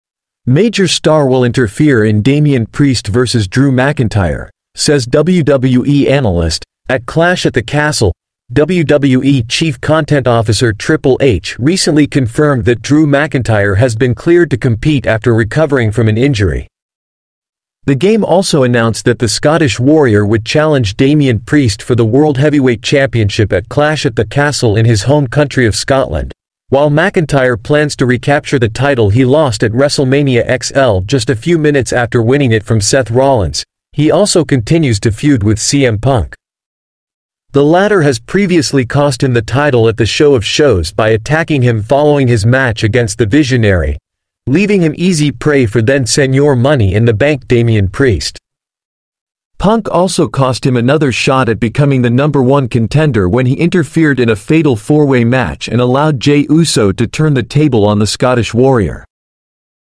Major star will interfere in Damian Priest vs. Drew McIntyre, says WWE Analyst, at Clash at the Cast
Major star will interfere in Damian Priest vs. Drew McIntyre, says WWE Analyst, at Clash at the Castle